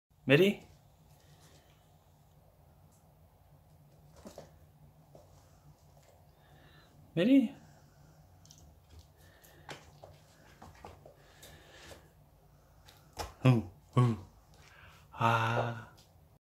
이름 알려주기 메리♡
안뇽하세요 푸들푸들이에요 오늘은
이름부르고 눈이마주치면 간식주기했어용 ♡
교육하는 영상은 찍지못했네요 ㅜ

귀여운 우리메리 활발해서 귀여워용 ㅋㅋ